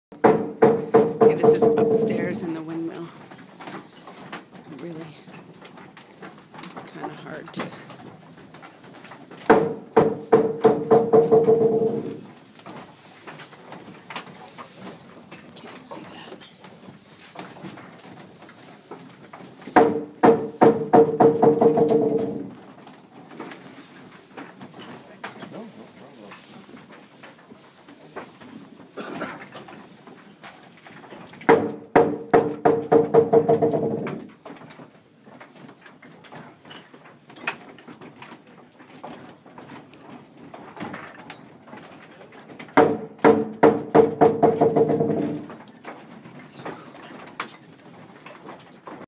Dutch windmill
Upstairs at the peanut oil windmill.